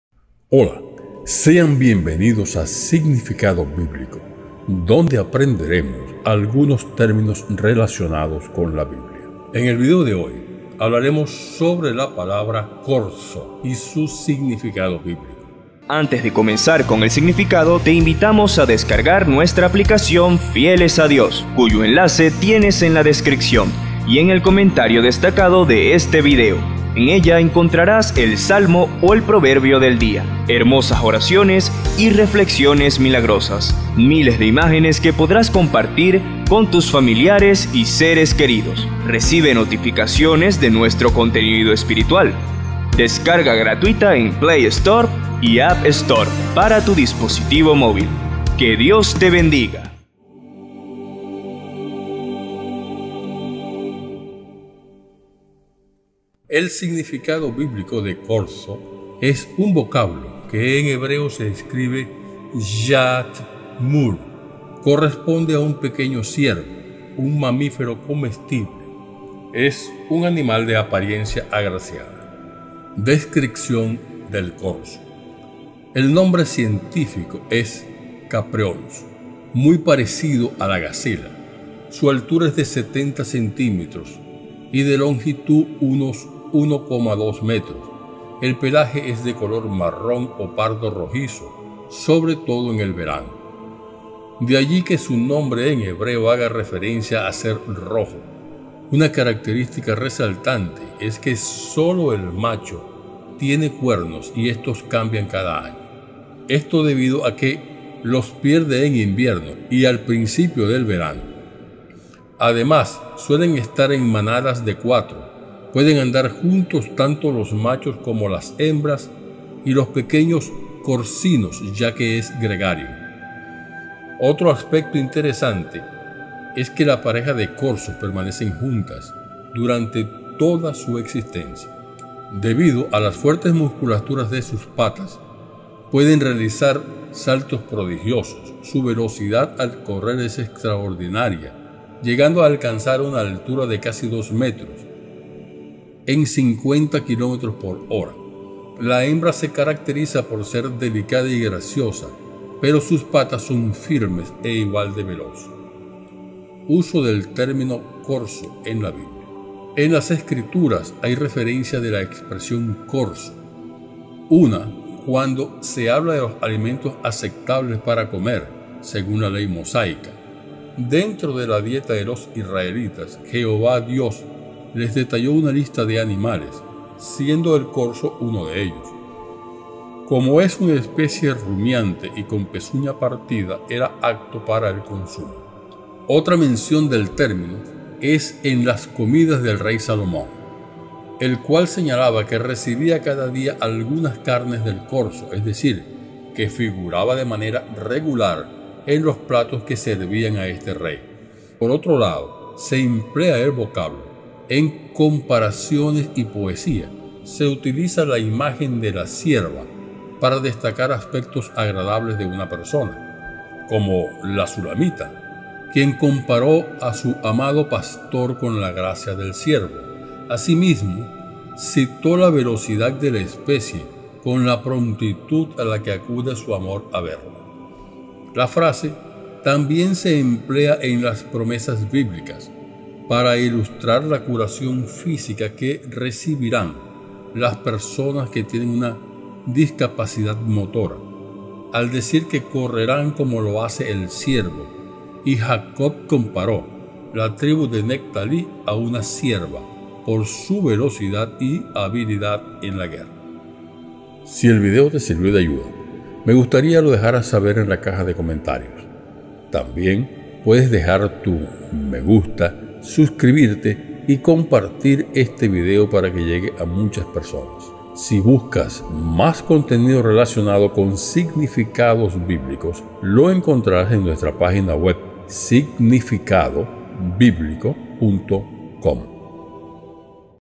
Corzo Significado Bíblico | ¿Qué Significa Corzo en la Biblia? 🙏
Aquí en @Significado Bíblico os compartimos el significado de Corzo, un termino muy interesante.

Bendiciones